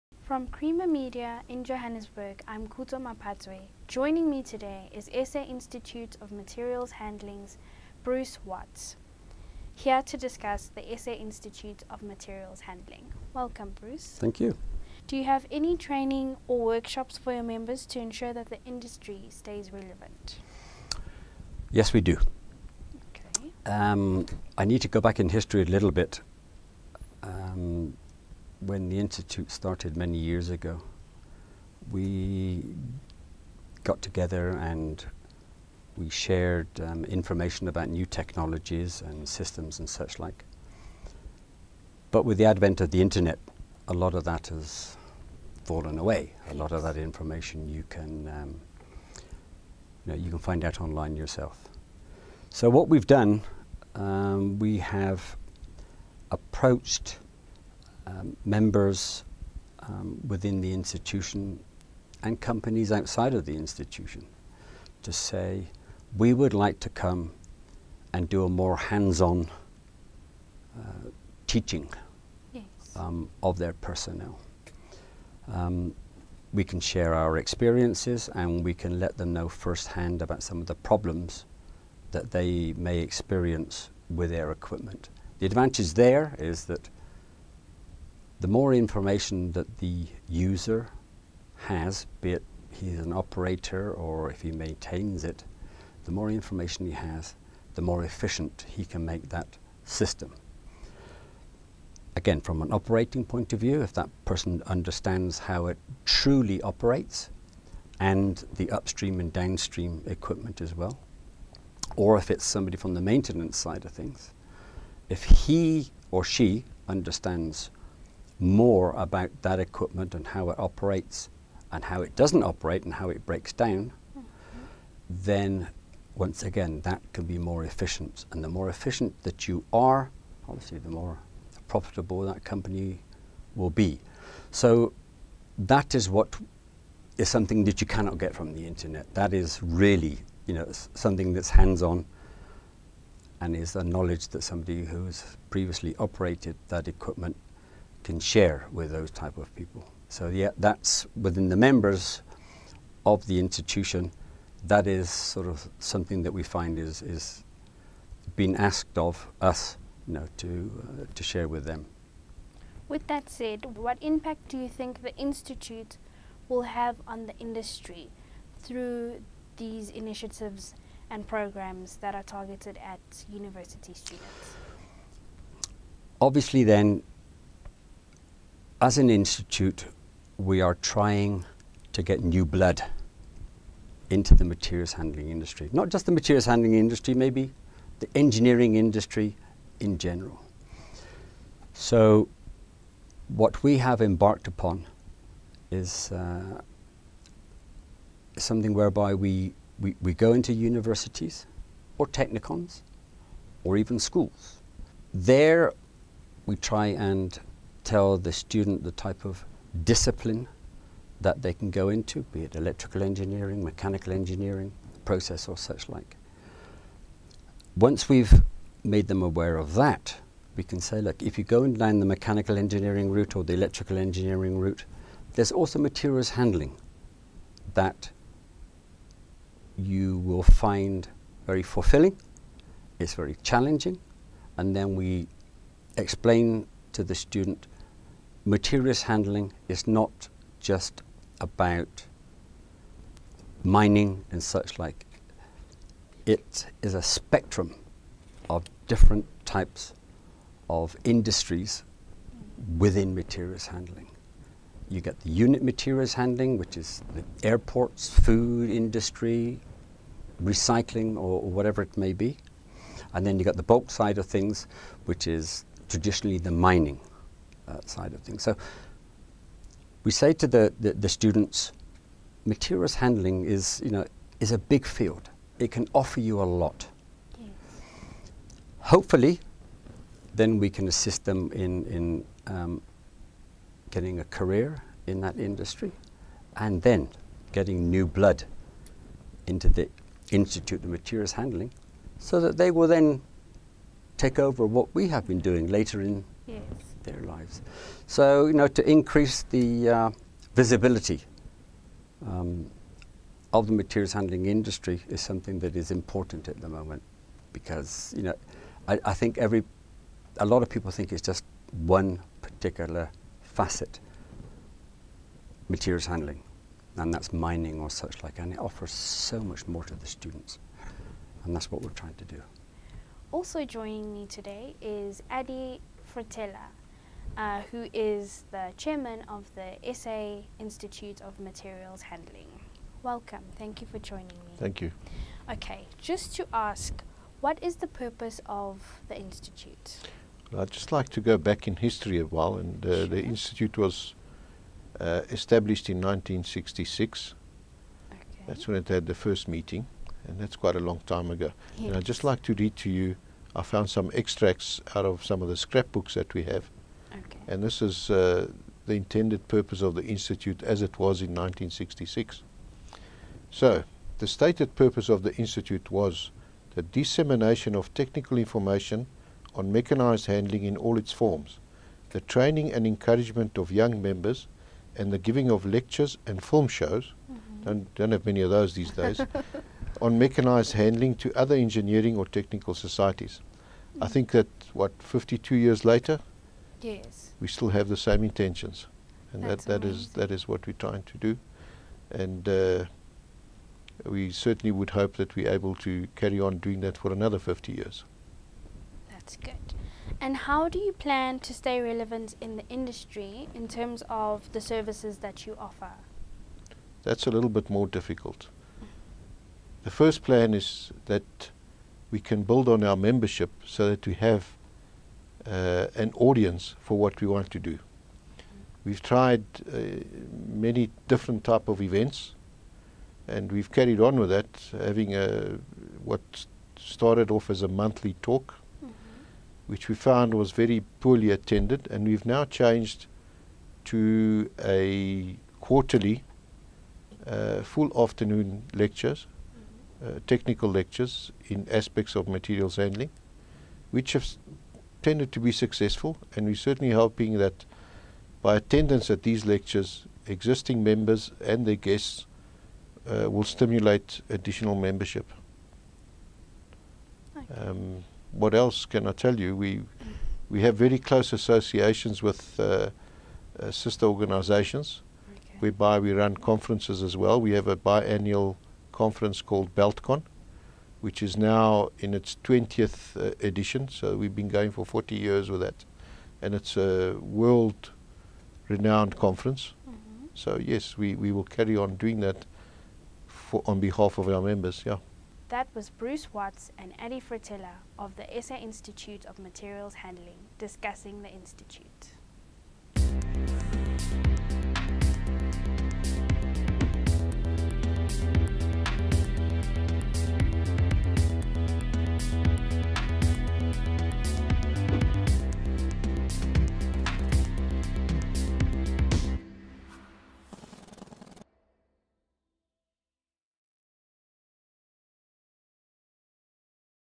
Education at forefront for materials handling institute
The committee of the South African Institute of Materials Handling (SAIMH) aims to reverse the trend of constantly declining institute membership by actively running training workshops and lectures to educate members and people that are new to the industry about the industry.